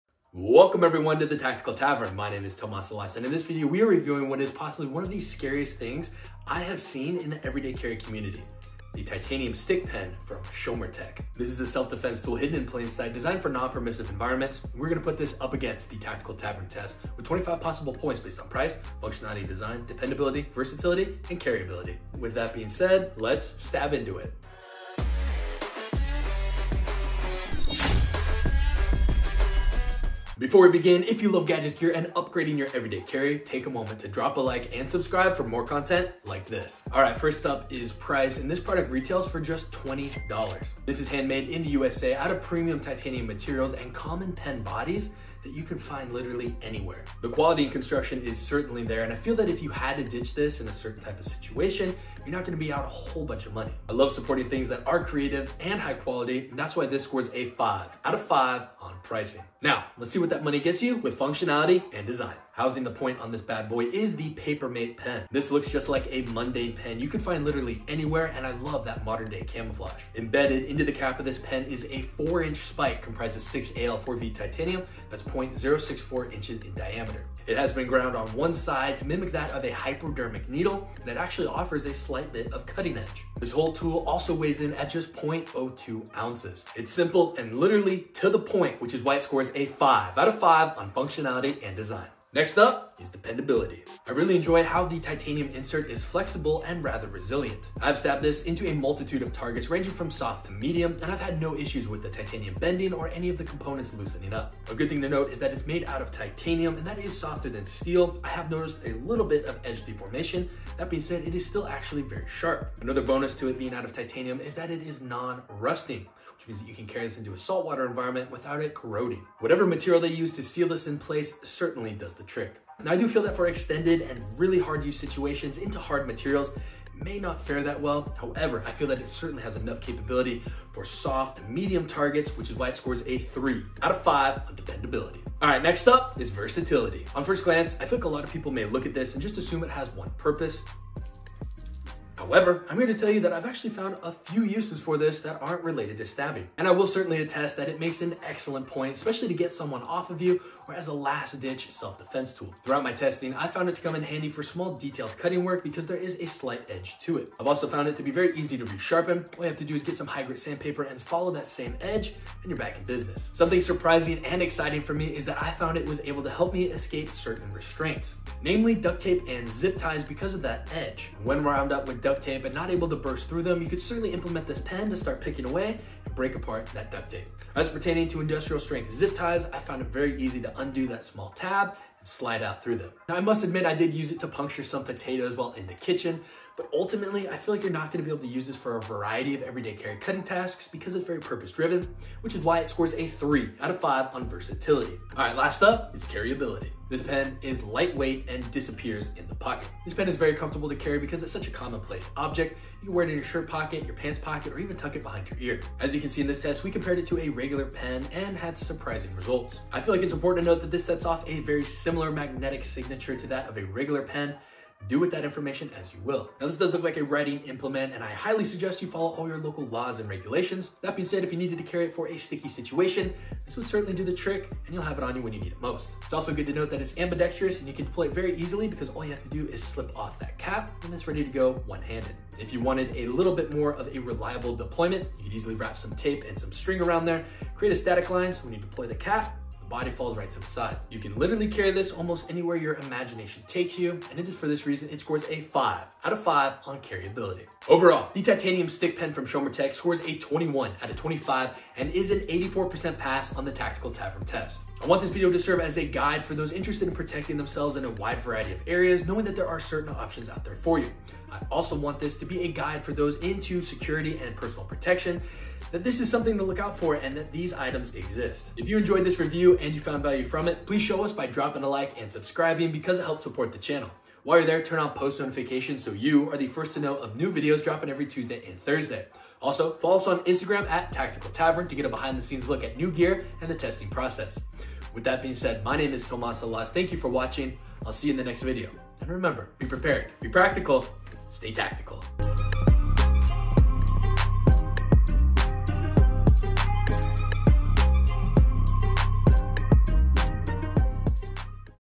SCARIEST Self Defense Tool for EDC | Titanium Stick Pen | Low Profile
Have you ever heard the term the pen is mightier than the sword? Well, in a close quarter combat situation I can certainly see this taking the cake!

This is the Titanium Stick pen from Shomer-Tec. The beauty of this pen is that it looks like an ordinary writing implement and even though it won't physically write, it still can draw up a lot of pain for an assailant.

In the past we have tested out other pens for everyday carry such as the Nightcore Ntp-31, Hobo Tactical penetrator,  carbon fiber ventilator, The Necro Maker Sharpie,  Tactile Turn slim bolt action pen, and The 5.11  double duty tactical pen. All of these pens mentioned above will be linked below to their respective reviews however none have been at Sinister in my opinion as this titanium stick pen. It is literally like a hypodermic needle and it is crazy Sharp.

We are going to put this up against the Tactical Tavern Test with 25 possible points based on Price, Functionality/Design, Dependability, Versatility, and Carry Ability.

Be Prepared, Be Practical, & Stay Tactical! ￼￼